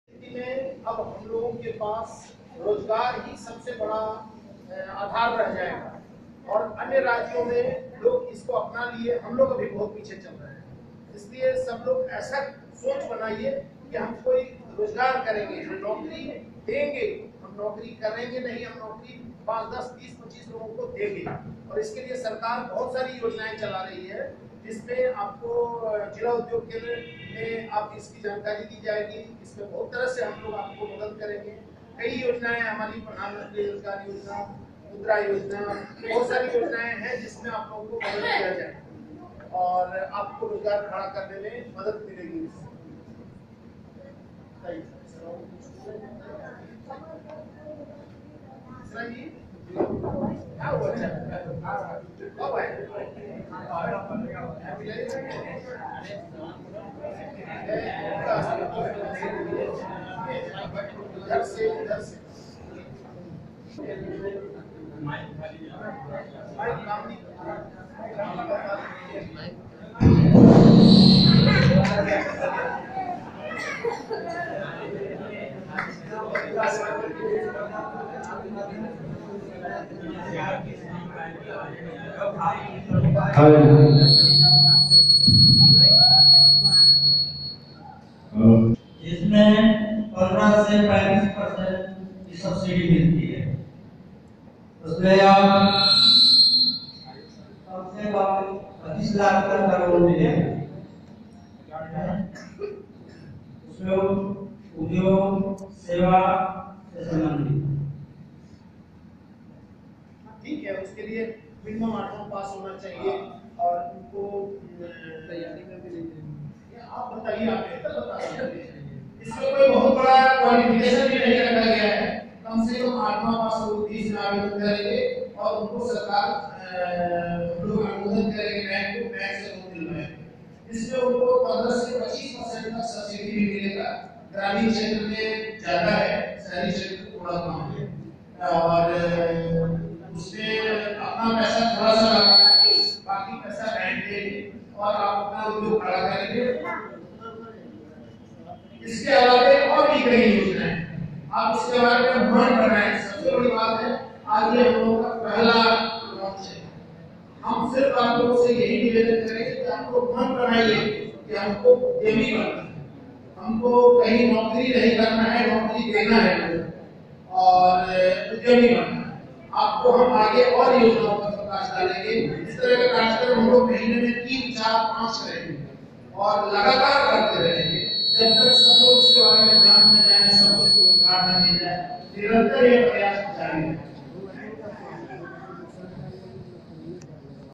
Seminar for New Entrepreneur (Business Person)
नए उद्यमियों के लिए उद्योग स्थापित करने हेतु सेमिनार, कैसर शकील